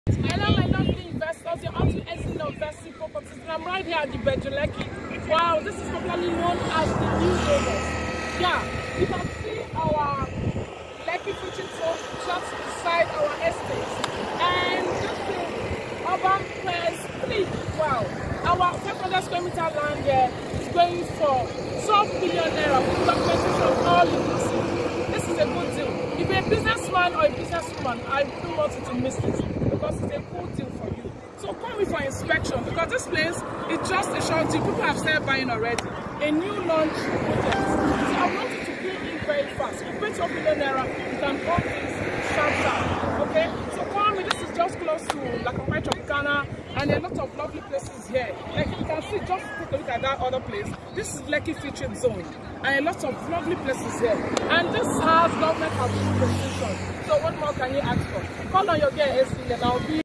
Urban Crest 3
Are you on the look out for a commercial plots to buy at Ibeju-Lekki, then welcome to Urban Crest 3. Its beside Lekki trade zone and its facing the express road, so don't miss this amazing deal guys.

Location  : Ibeju-Lekki Lagos

Price : N12m per plot